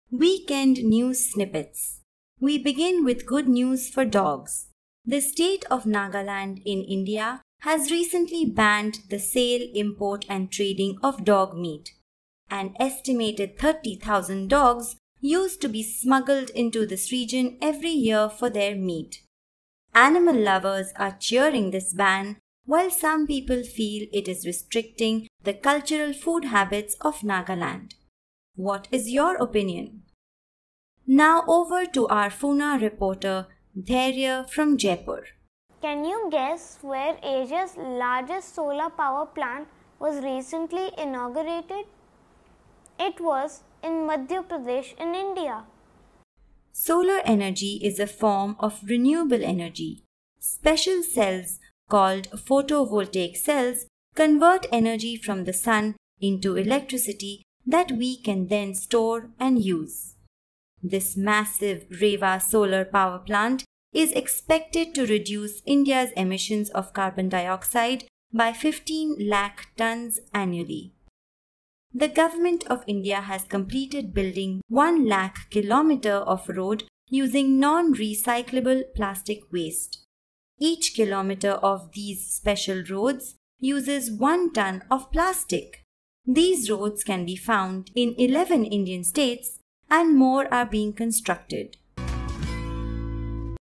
Weekend News Snippets From India | Animal Lover Cheer | Asia’s Largest Solar Plant | Current Affairs
Interesting news from India. The country gets Asia’s largest solar power plant in Rewa, Madhya Pradesh and is using plastic waste to build roads. In some good news for animal lovers another state in India has banned dog meat.

Many more videos like this are available on our app 'Foona'